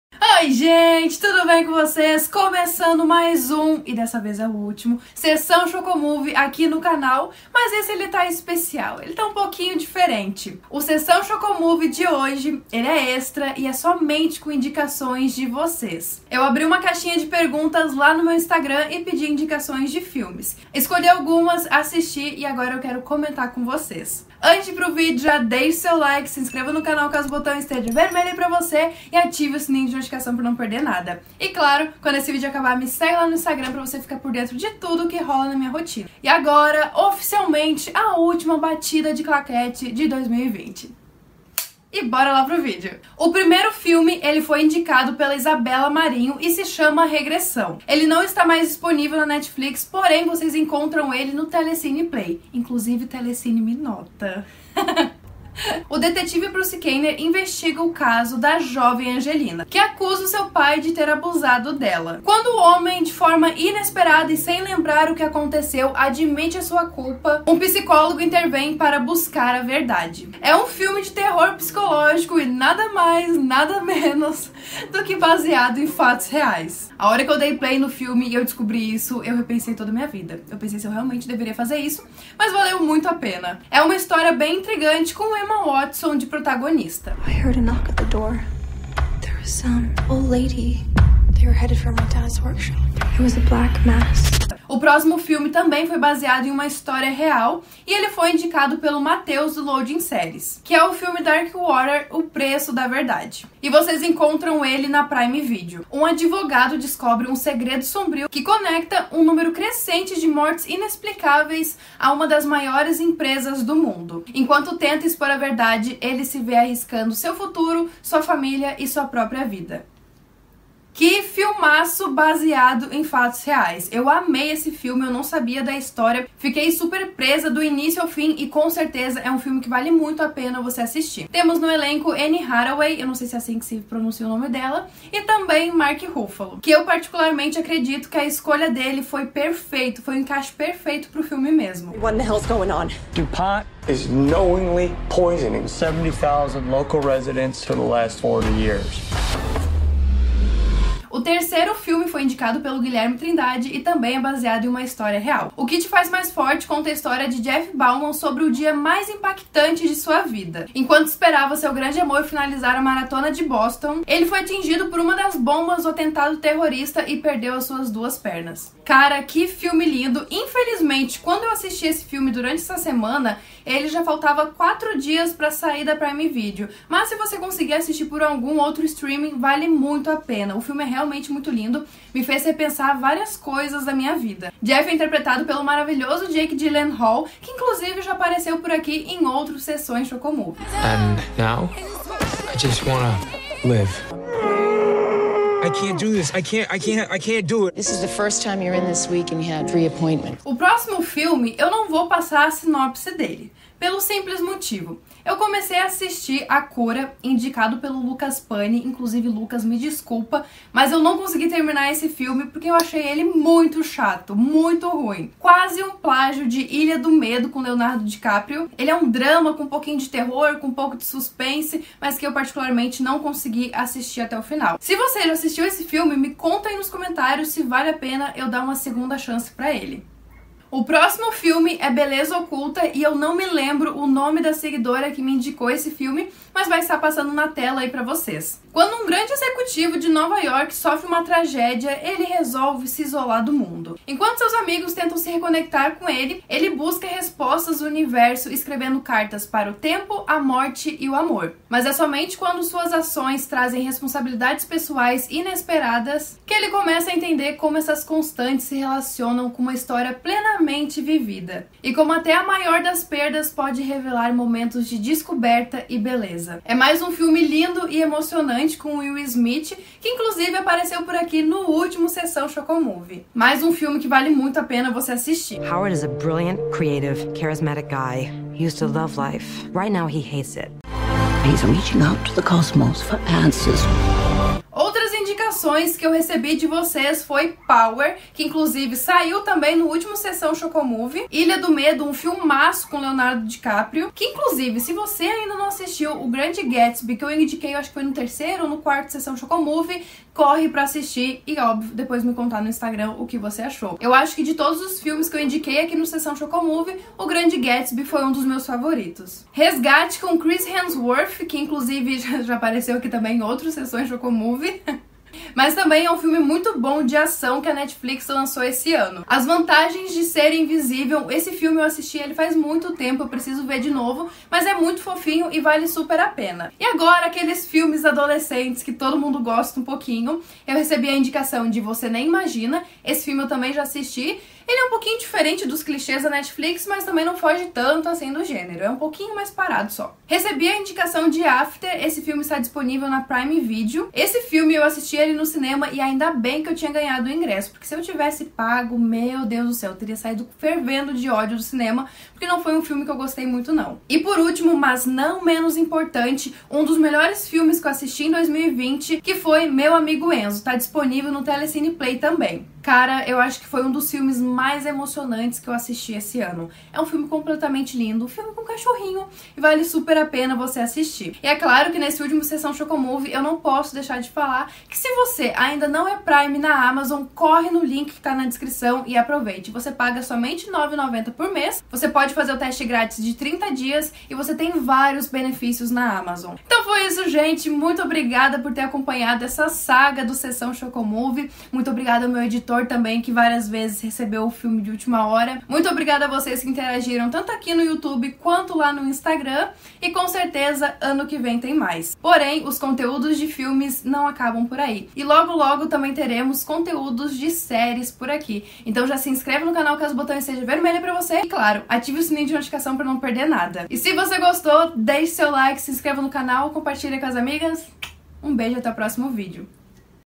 ASSISTI OS FILMES QUE VOCÊS RECOMENDARAM SessãoChocoMovie EXTRA | Agatha Nogueira
Um Sessão ChocoMovie somente com as indicações de vocês! Abri uma caixinha no Instagram e selecionei alguns filmes que vocês indicaram para eu assistir. Tem dicas de filmes na Netflix, Prime Video e também Telecine Play, desde baseados em história real, suspense, terror e claro, aquele romance teen que não pode faltar. Espero que gostem, um beijo e até o próximo vídeo!
TESTE AMAZON PRIME VIDEO: